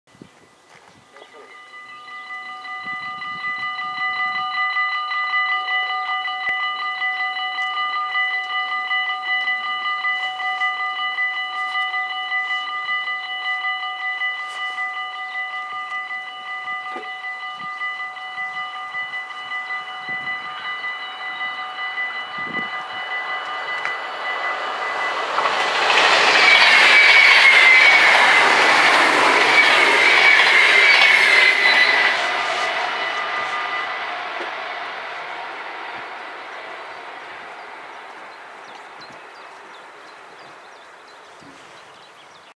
Spoorwegovergang Spankeren // Dutch railroad crossing
Locatie: Overweg
Traject: Zutphen - Arnhem
Soort: Mini-AHOB
Rode lichten: 8
Bellen: 2
Bomen: 2
Andreaskruisen: 2
Passeren:
- ICMm richting Roosendaal